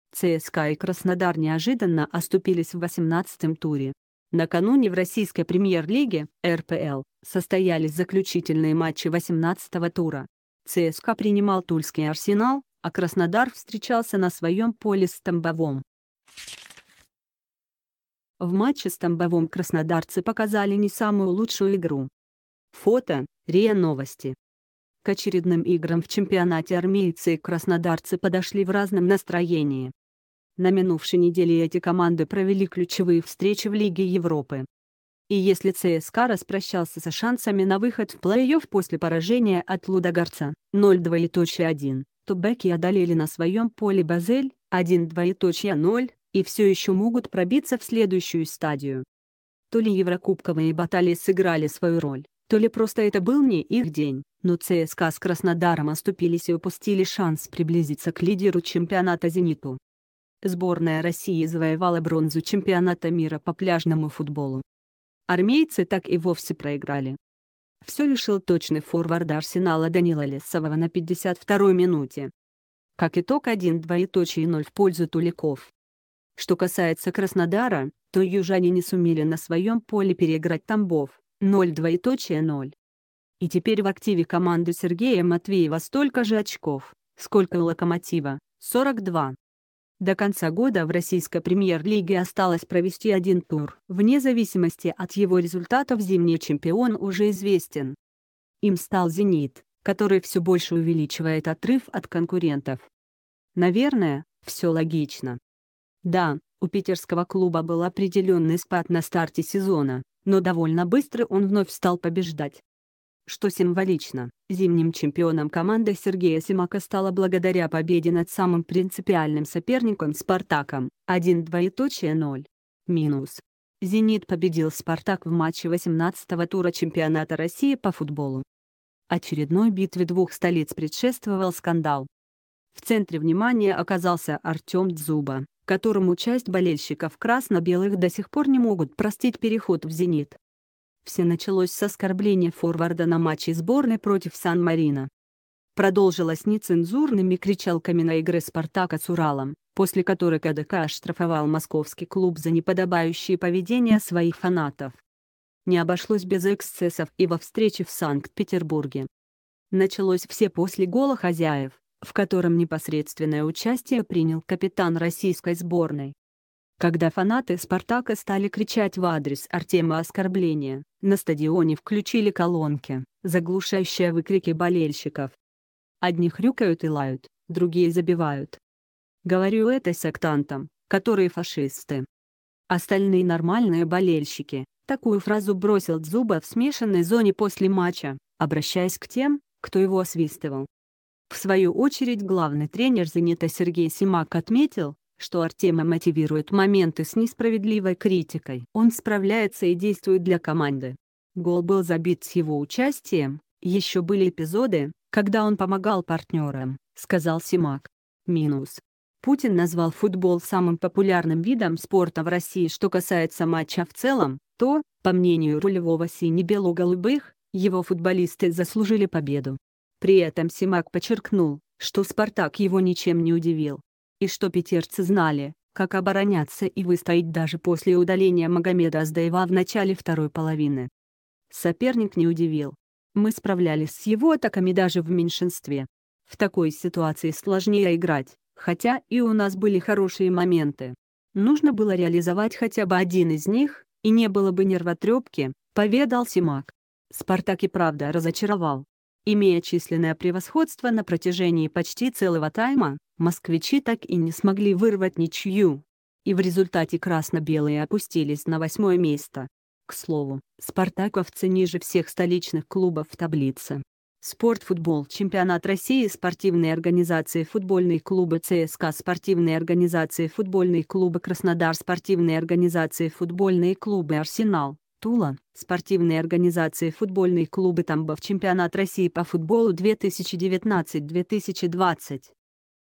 ЦСКА и Краснодар неожиданно оступились в 18-м туре
ЦСКА и Краснодар неожиданно оступились в 18-м туре Накануне в российской Премьер-лиге РПЛ состоялись заключительные матчи 18-го тура . ЦСКА принимал тульский Арсенал , а Краснодар встречался на своем поле с Тамбовом . В матче с Тамбовом краснодарцы показали не самую лучшую игру . Фото РИА Новости К очередным играм в чемпионате армейцы и краснодарцы подошли в разном настроении . На минувшей неделе эти команды провели ключевые встречи в Лиге Европы . И если ЦСКА распрощался с шансами на выход